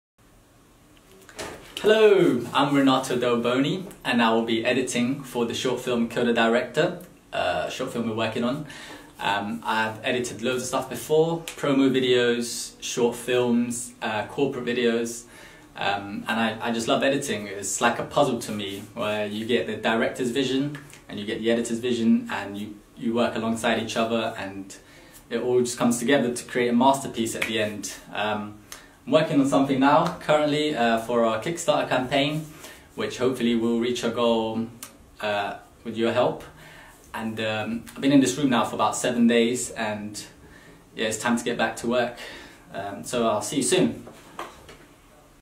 ComRom Meet Renato Delboni!
Meet the Lead Editor of Kill the Director: Renato Delboni!

'Kill The Director' is a short comedy romantic film that tells the story of a young failed romantic named Toby, who attempts to woo a girl named Zoe by watching loads of 'rom-com' films, but instead becomes trapped in a 'rom-com' universe himself...